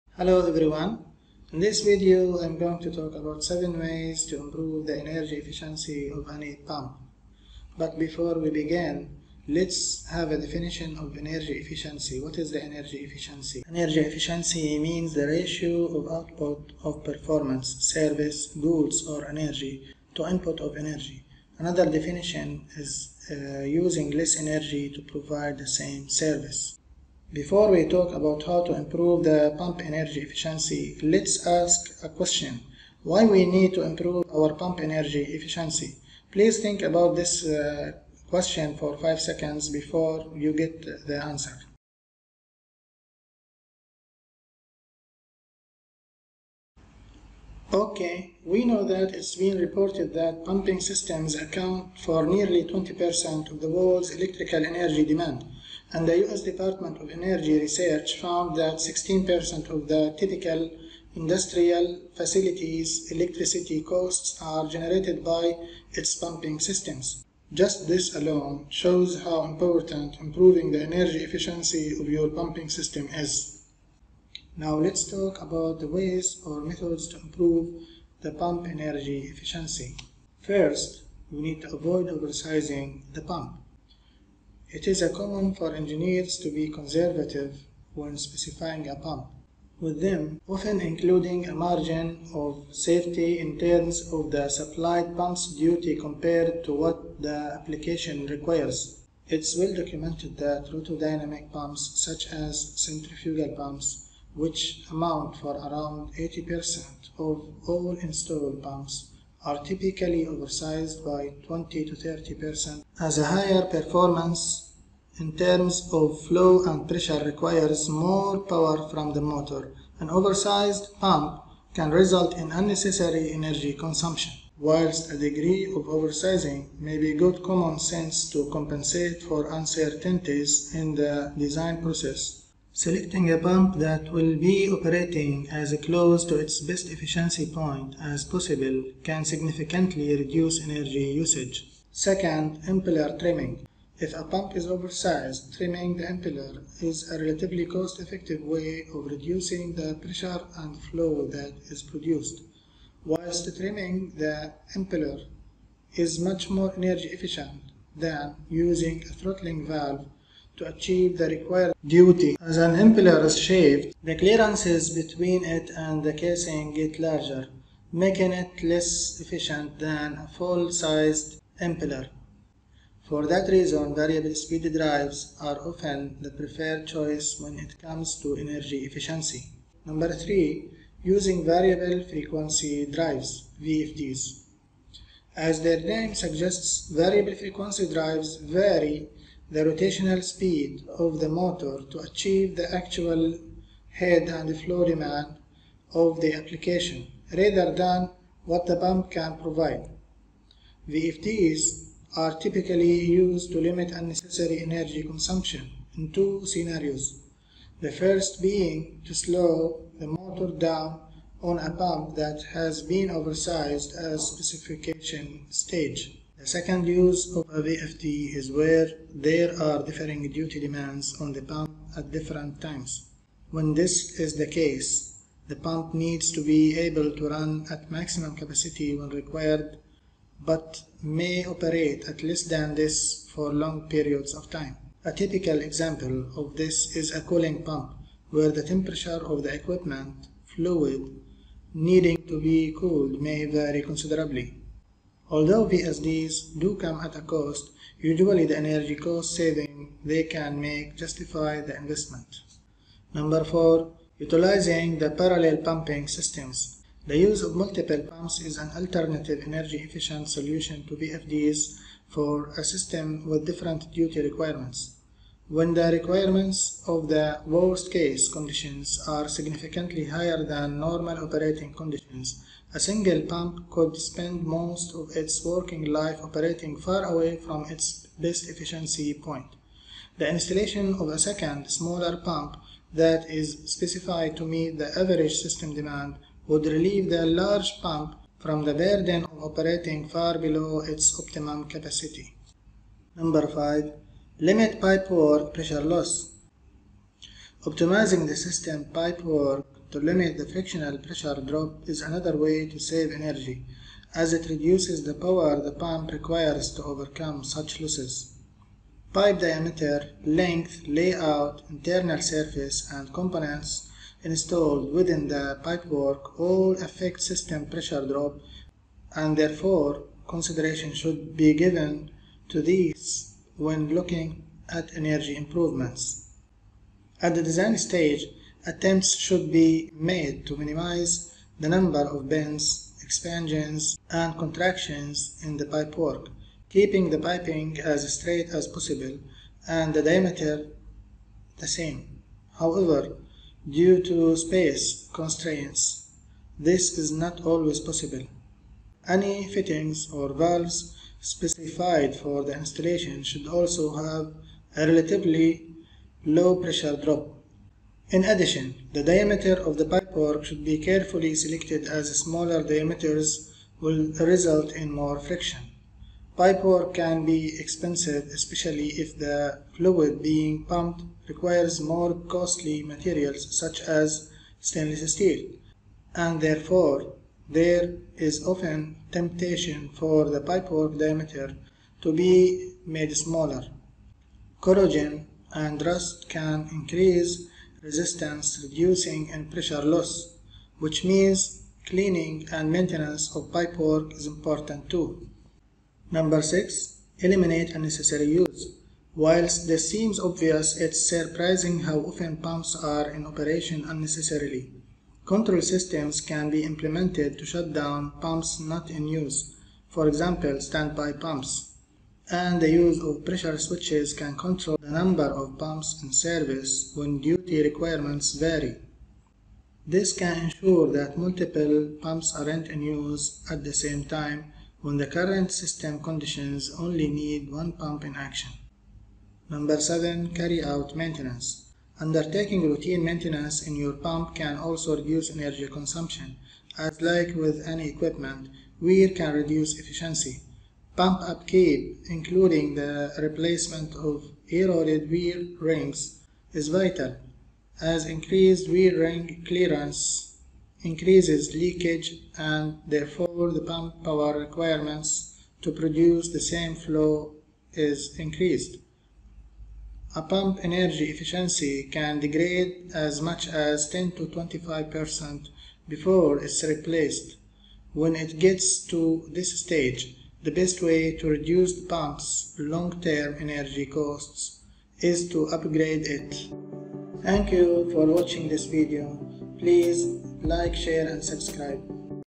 How to Improve Pump Energy Efficiency
0:00 Introduction
0:09 What is Energy Efficiency?
0:27 Why we need to improve our pump energy efficiency
1:22 1. Avoid Over-sizing the pump
2:22 2. Impeller Trimming
3:00 3. Use Variable Frequency Drives
4:13 4. Parallel Pumping Systems
4:52 5. Limit Pipework Pressure Loss
6:36 6. Eliminate Unnecessary Use
7:09 7. Carry out Maintenance

Improve the Energy Efficiency of Pump Systems
Pumping systems can be critically important to
a plant’s operations. In many industrial applications, such as power and petrochemical plants, pumps directly support production processes and run as often as—or even longer than—any other equipment at the facility. The amount of energy consumed by many long-running pumping systems often results in a substantial addition to
a plant’s annual operating costs. In fact, about 27% of all the energy consumed by motor-driven equipment in manufacturing facilities is used to operate pumps.3 Therefore, pumping systems
are a natural target in efforts to reduce energy consumption in motor-driven systems.
In some cases, pumping system energy is used quite efficiently; in others, it is not. Facility operators are often very familiar with the controllability, reliability, and availability of pumping system equipment, but they might not
be as aware of system efficiency issues—and there are good reasons to increase their awareness. For example, there is a strong correlation between the reliability of pumps and their efficiency; that is, pumps that operate close to their best efficiency point tend to perform more reliably and with greater availability.